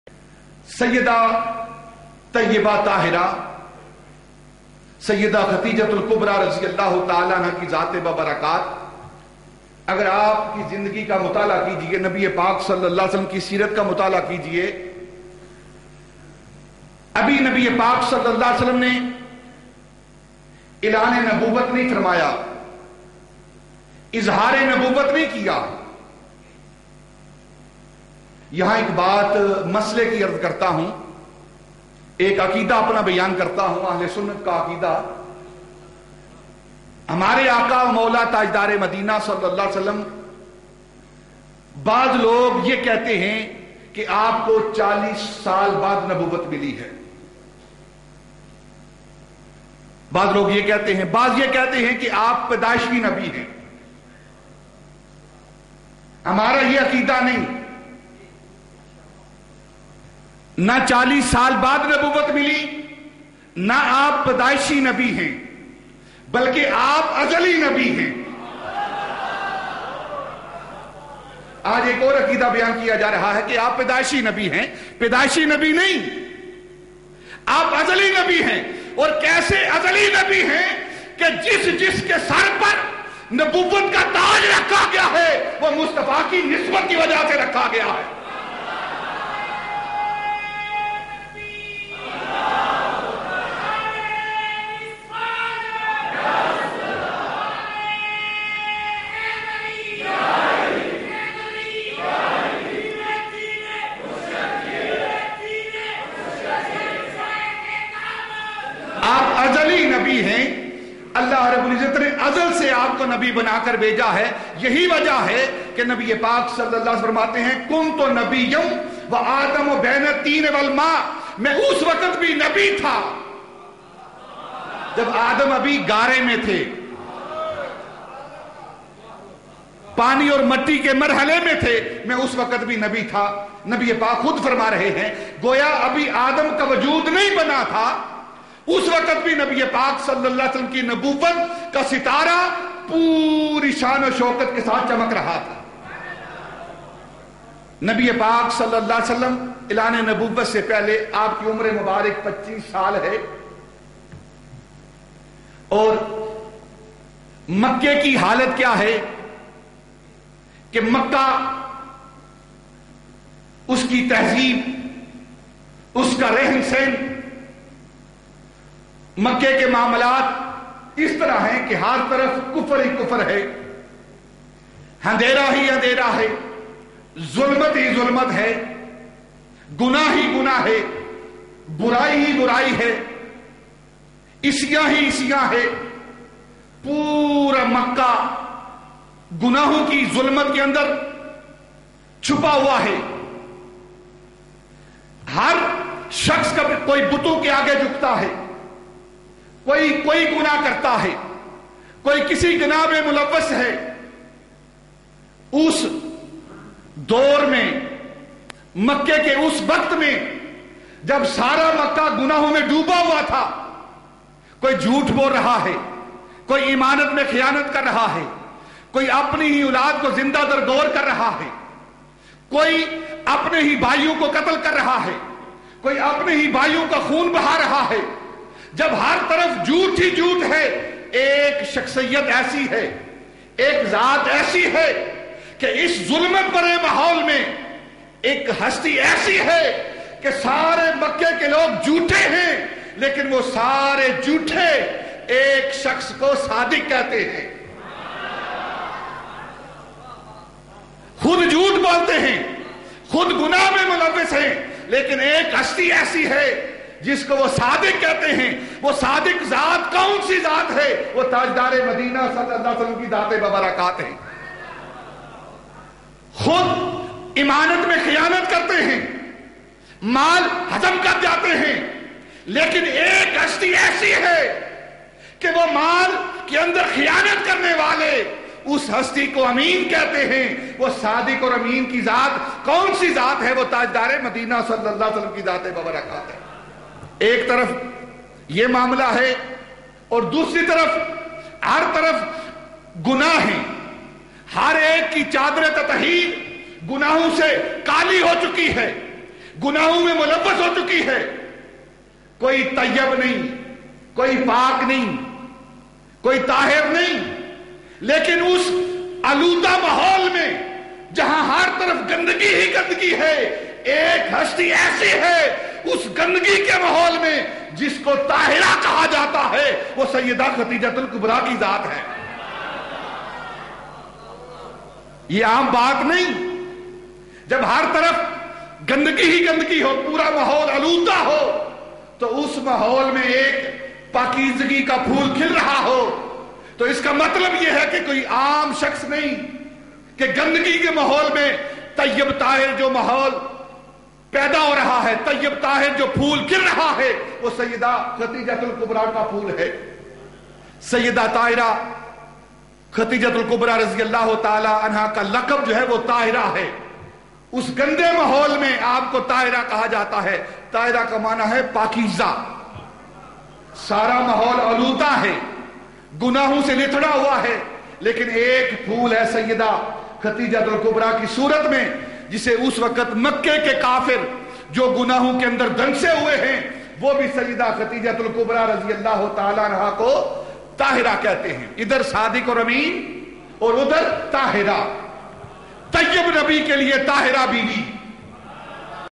سیدہ خدیجۃالکبری کی شان طہارت، ابوالحبیب پیرمحمد رفیق احمد مجددی۔ Syeda Khadija Kubra ki Shane Taharat, Peer Muhammad Rafiq Ahmad Mujaddadi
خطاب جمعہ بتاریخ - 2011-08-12
موضوع "مومنہ اول سیدہ خدیجۃ الکبری  رضی اللہ تعالی عنہ" Momina Awwal Syeda Khadija Kubra